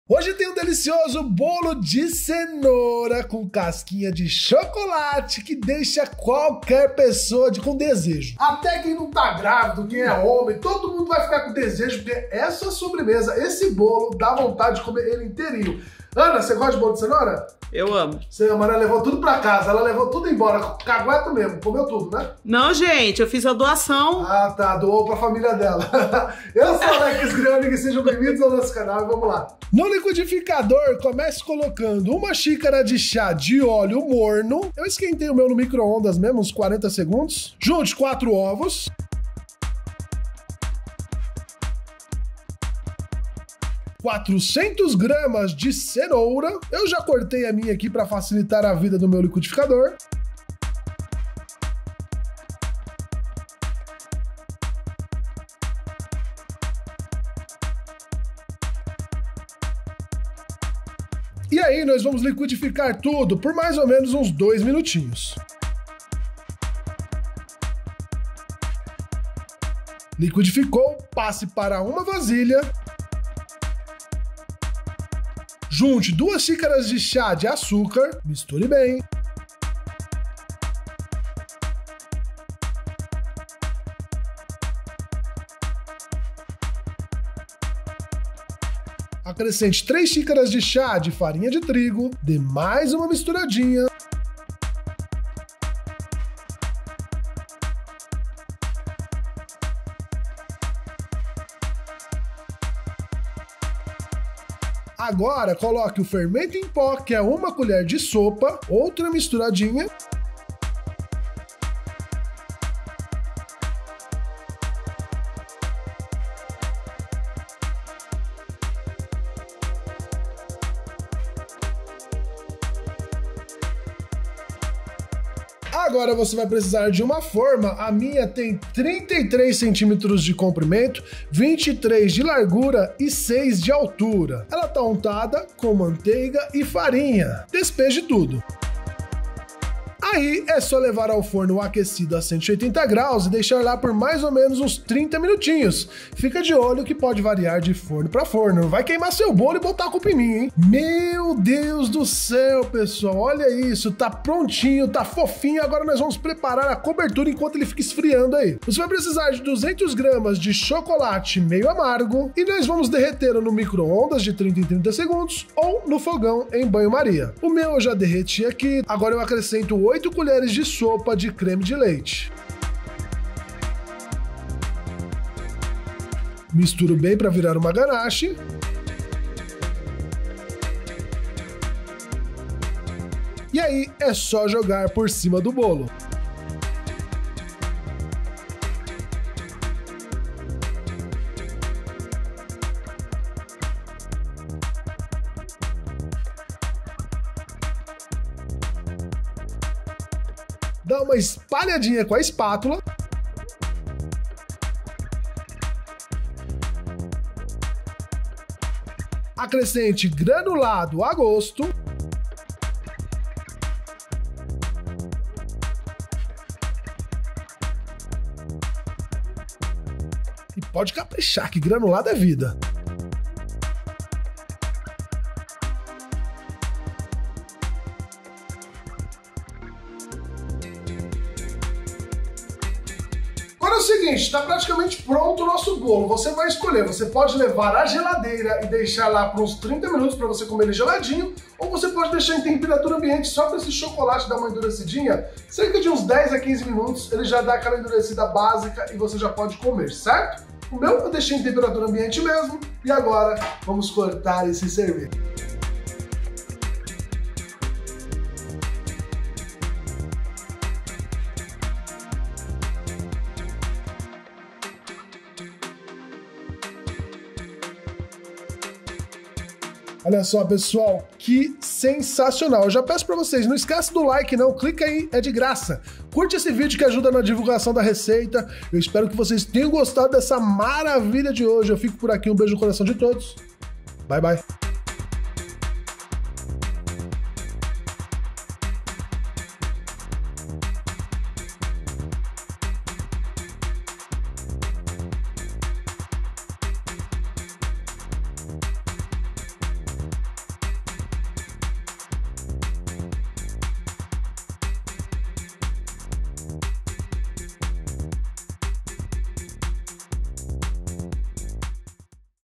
BOLO DE CENOURA COM CASQUINHA DE CHOCOLATE 😍
Bolo de Cenoura é bom demais, agora com uma casquinha deliciosa de cenoura fica melhor ainda! Hoje eu ensino como fazer essa receita =)

Ingredientes Bolo de Cenoura:

1 xícara (chá) de óleo morno
4 ovos
400g de cenoura
2 xícaras (chá) de açúcar
3 xícaras (chá) de farinha de trigo
1 colher (sopa) de fermento em pó

Ingredientes Cobertura:

200g de chocolate meio amargo ou ao leite pode por 100g de cada se quiser
8 colheres (sopa) de creme de leite
granulado a gosto

Medidas do Canal:

1 xícara (chá) = 240 ml
1 copo americano = 100 ml
1 lata de leite condensado = 395g
1 caixinha de creme de leite = 200g
1 vidrinho de leite de coco = 200 ml